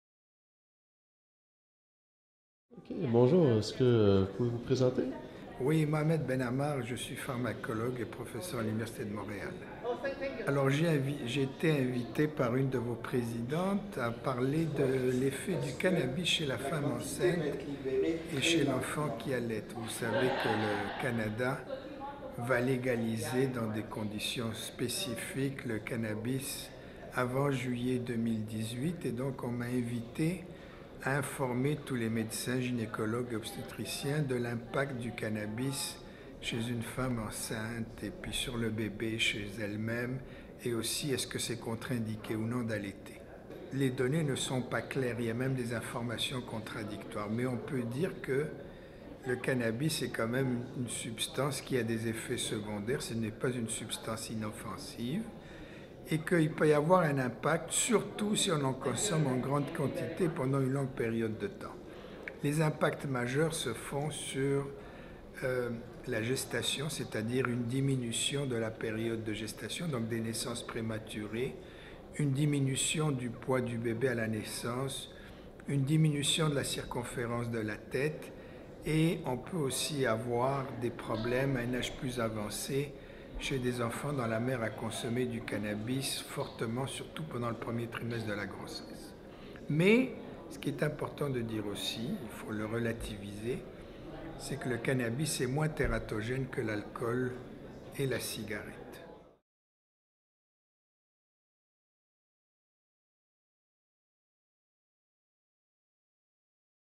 Mohamed Ben Amar parle de la consommation de cannabis pendant la grossesse
Bientôt, le cannabis deviendra légal au Canada. Quels sont les effets du cannabis sur les femmes enceintes et leur enfant à naître?

Mohamed Ben Amar, pharmacologue, nous parle de ce que nous savons à l’heure actuelle au sujet des effets de la consommation de cannabis pendant la grossesse et l’allaitement.